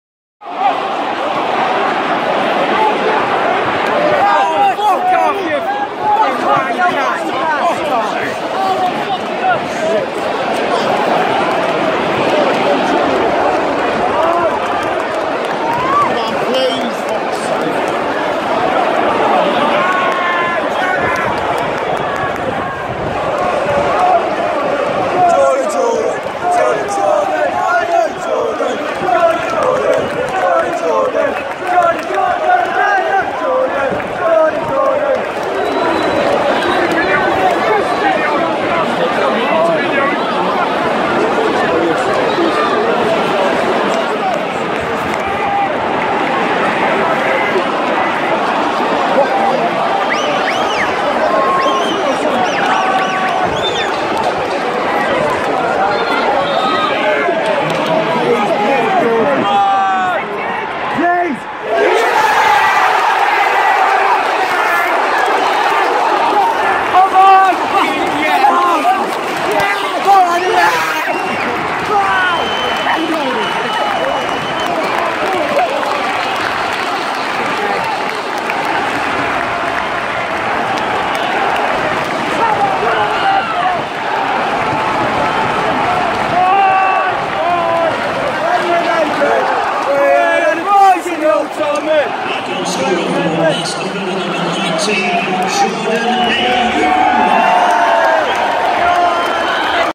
Jordan Ayew scores magical last-minute winner that earns him comparisons with Leo messi
How Messi is THAT?! Ayew scores magical last-minute winner that earns him comparisons with